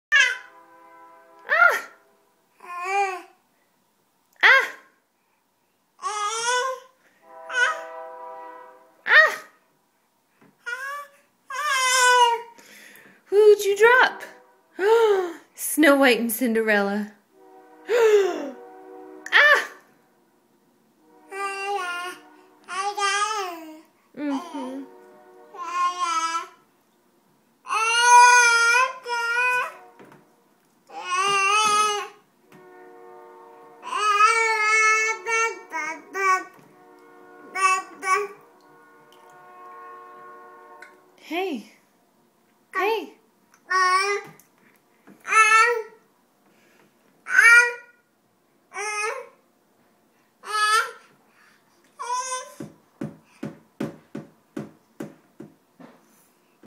Talking in the Bathtub
Mazie and mommy having a conversation.